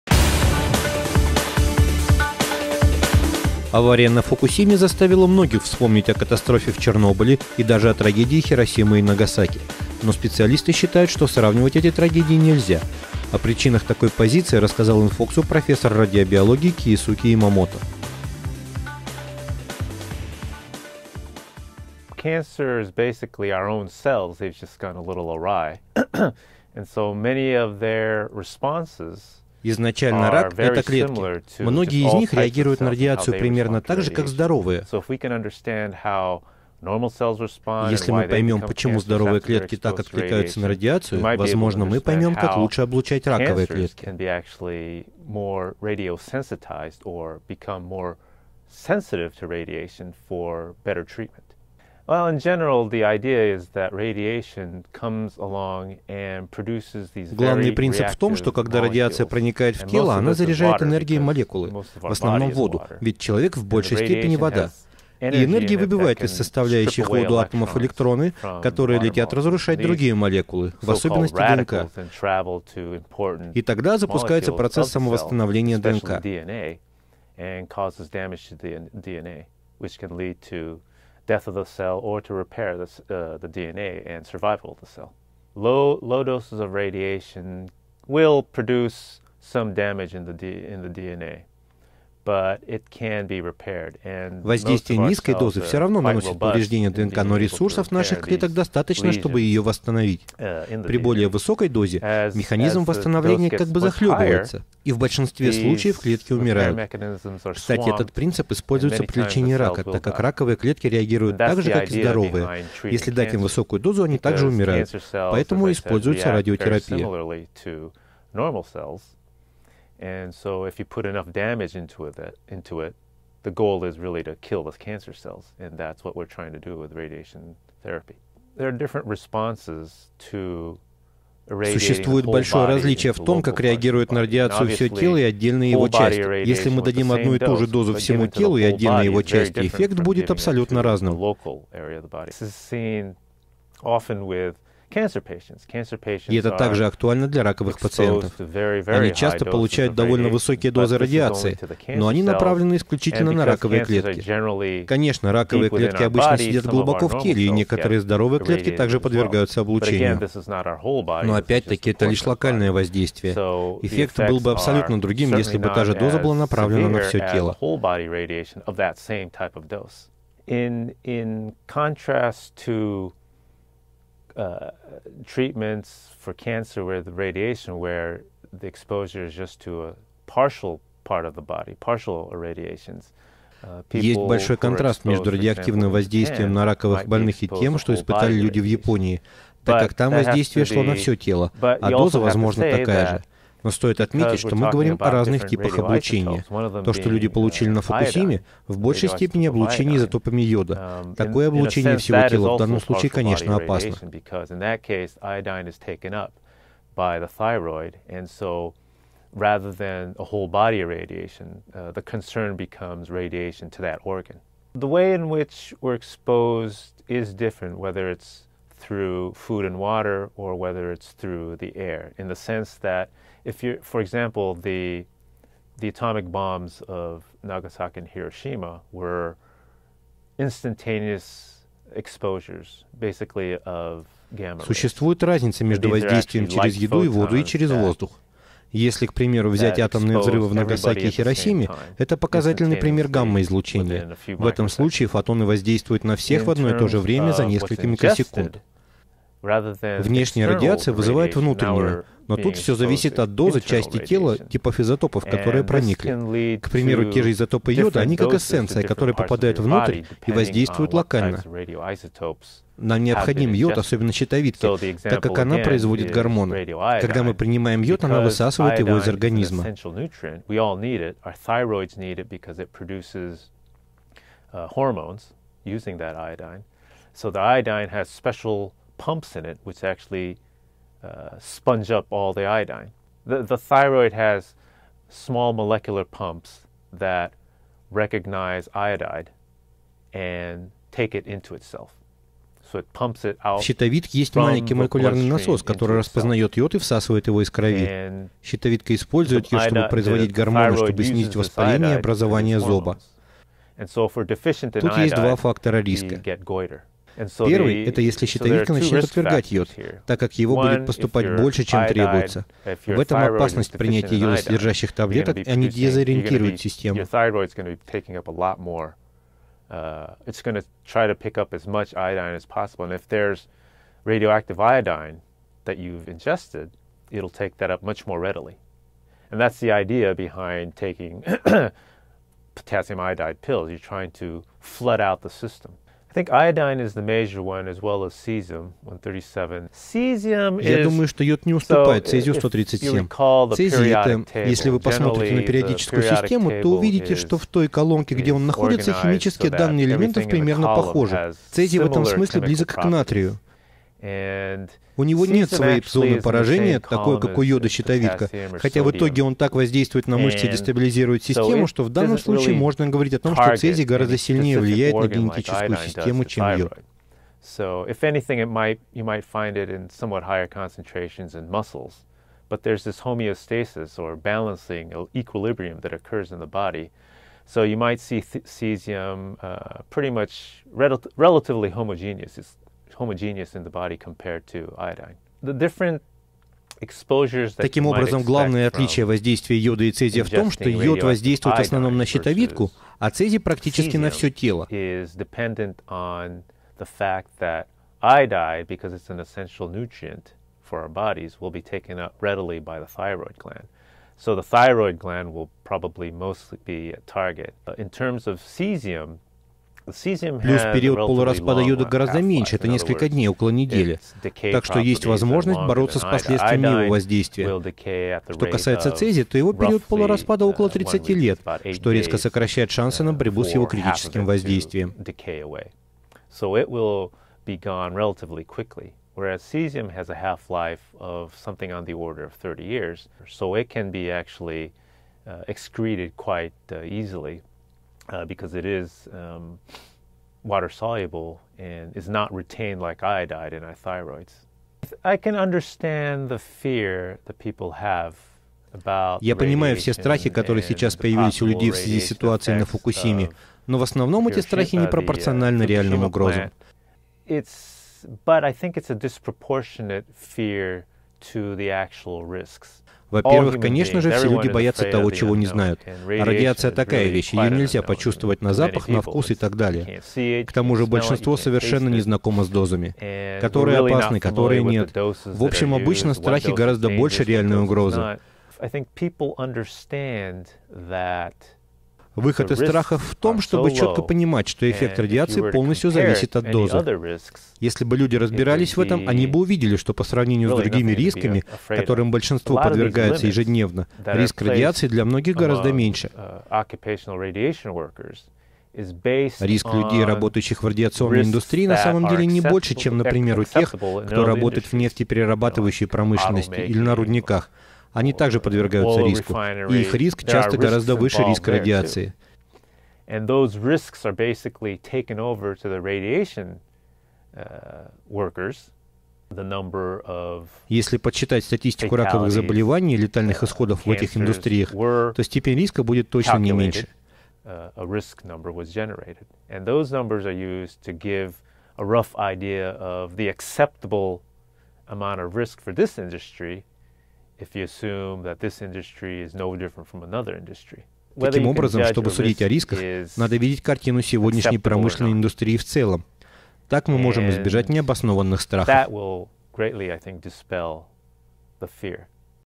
Радиобиолог: «Я думаю, что йод не уступает цезию-137»
Чем похожи, а чем отличаются трагедии Фукусимы, Чернобыля, а также Хиросимы и Нагасаки, к каким последствиям они приводят и чего следует опасаться, рассказал в интервью Infox.ru профессор радиобиологии Киисуки Иомото.
Дата публикации: 10 апреля 2011 13:44